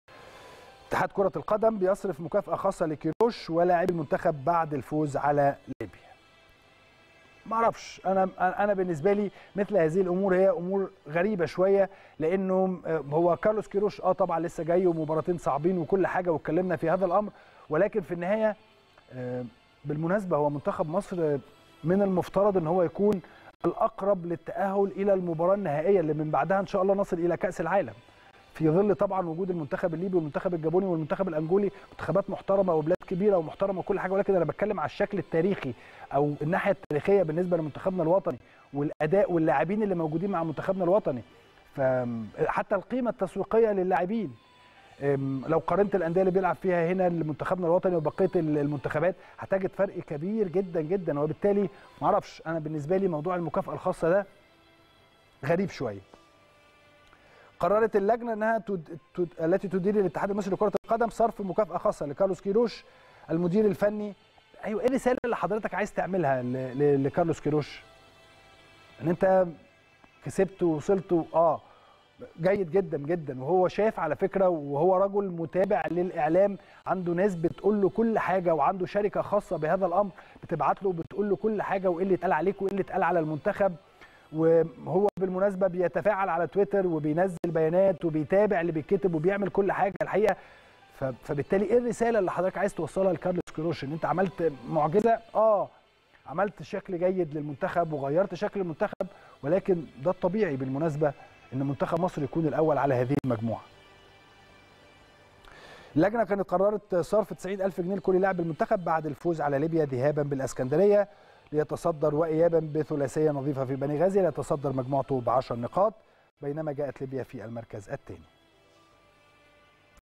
اتحاد كرة القدم يصرف مكافأة خاصة لـ"كيروش" ولاعبي المنتخب بعد الفوز على ليبيا.. إليكم التفاصيل.
اتحاد كرة القدم يصرف مكافأة خاصة لـ"كيروش" ولاعبي المنتخب بعد  الفوز على ليبيا.. إليكم التفاصيل | البيت الكبير